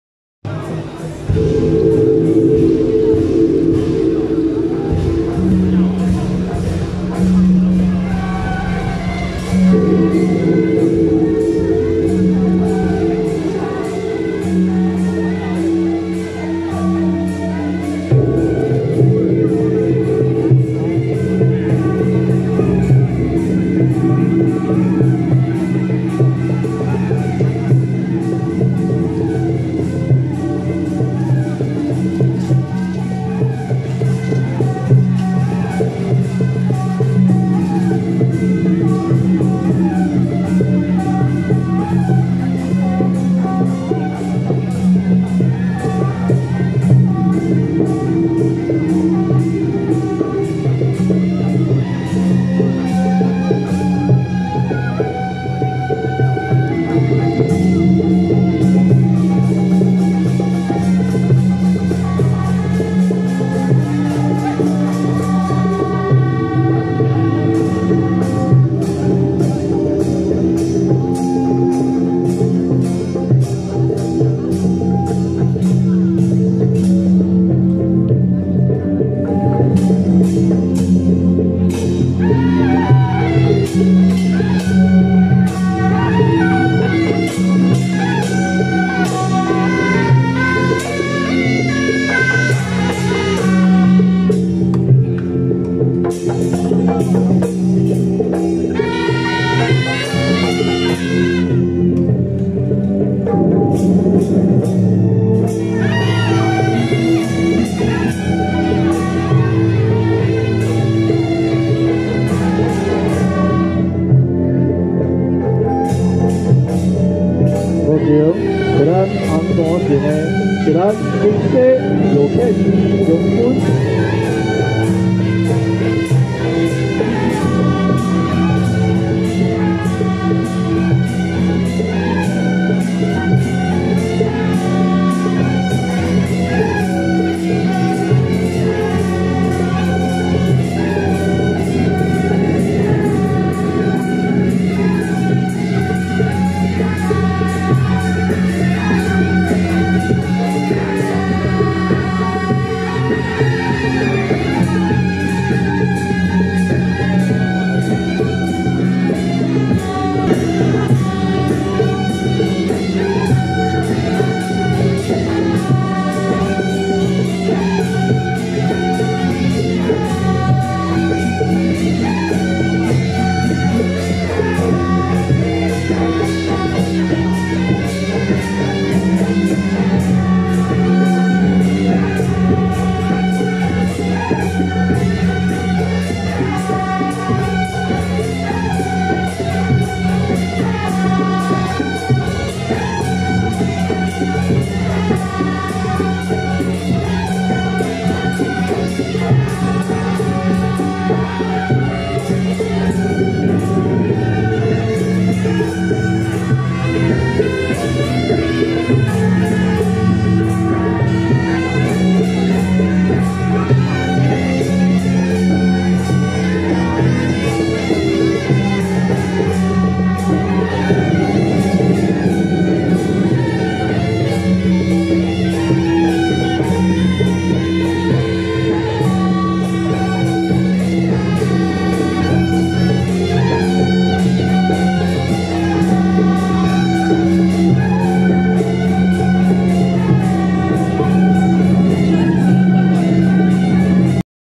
【嶺頂音聲社-北管＆桃園聯興社-神將團】~108龜山紫龍宮鎮殿觀音菩薩開光啟靈暨建宮10週年遶境
嶺頂音聲社-北管＆桃園聯興社-神將團